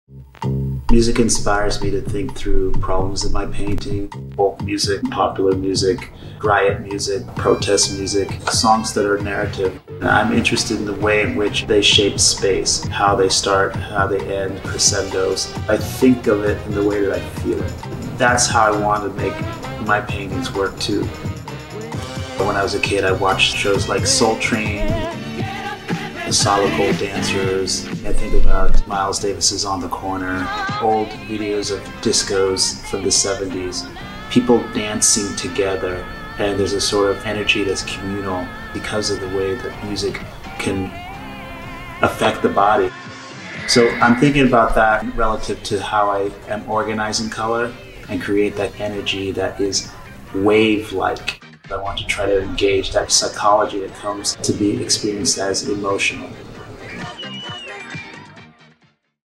Pew Fellows in Process: Abstract artist Odili Donald Odita
Abstract painter Odili Odita describes how music informs his use of color for our series “Pew Fellows in Process.”